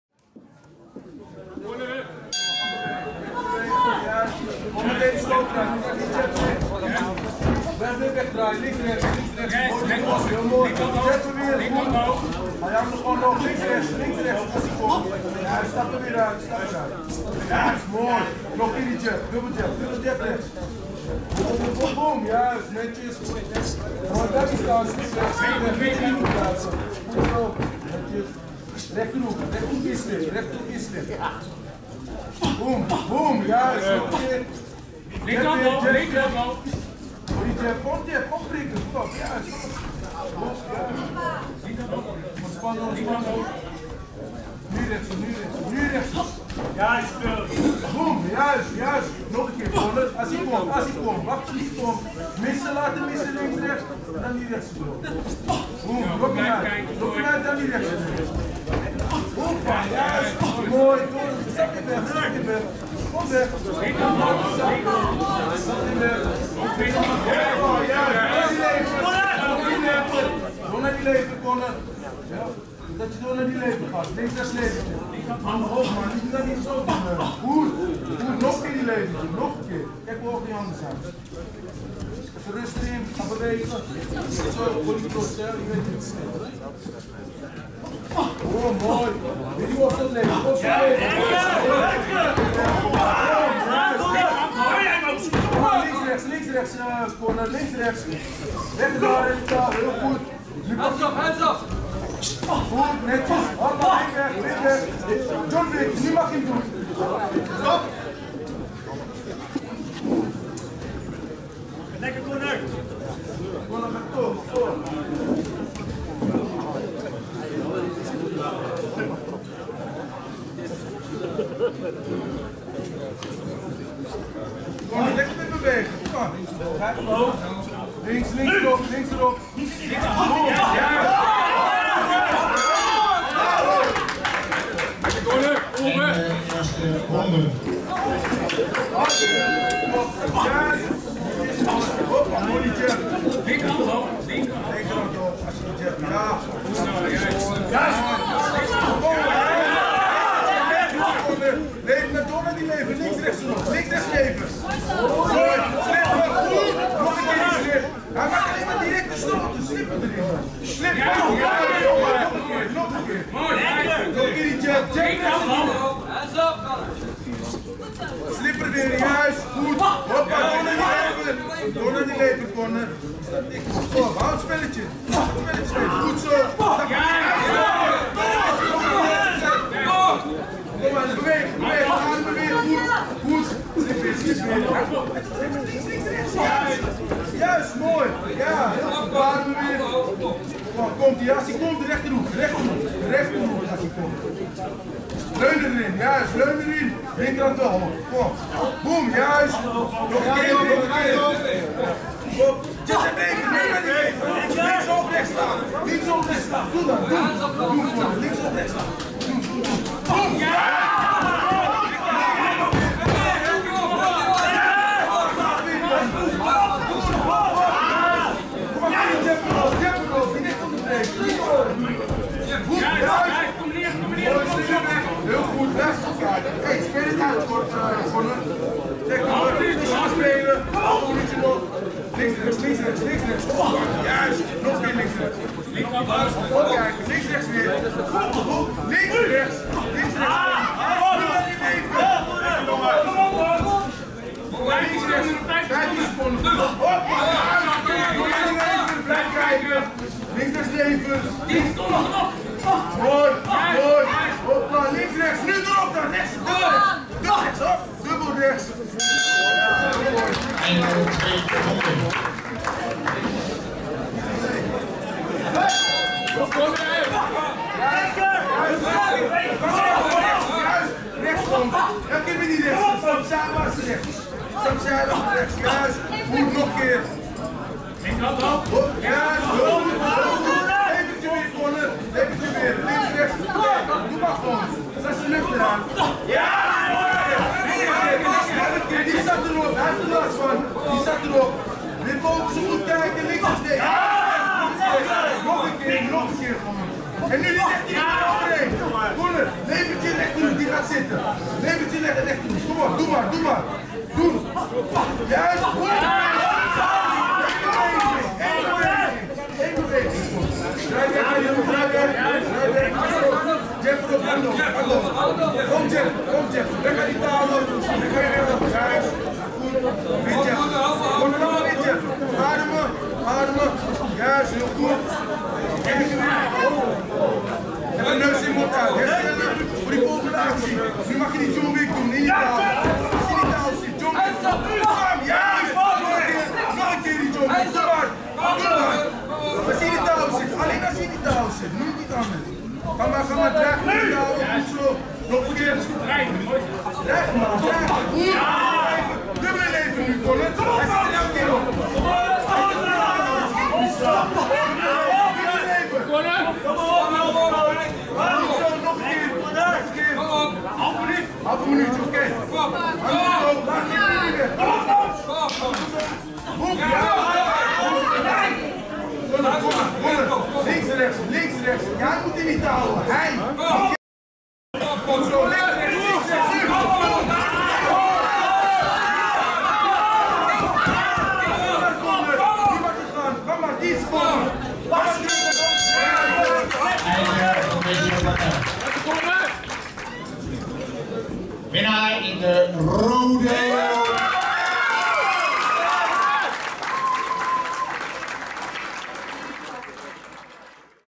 FIGHTCLUB DEN HAAG ON TOUR - Conner Byrne vs Mosa Kerim
Amateur boksen 3x2 minuten
Fightclub Den Haag On Tour - Oldskool 1.0